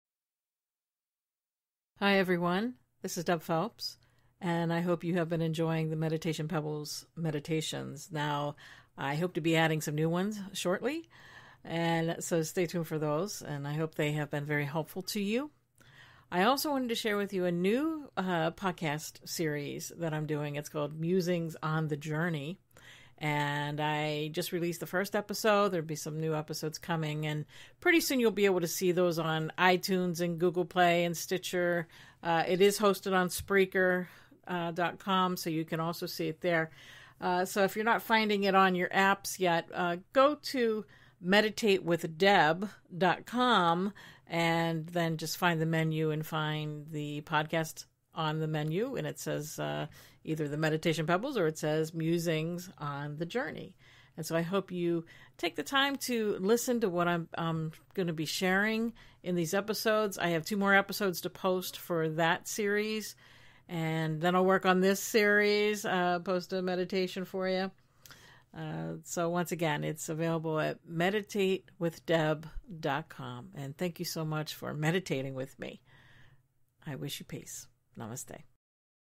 Special Announcement New Podcast Series
Check out Deb's new podcast series called "Musings on the Journey." It will be available soon on your usual podcast apps. and spreaker.com.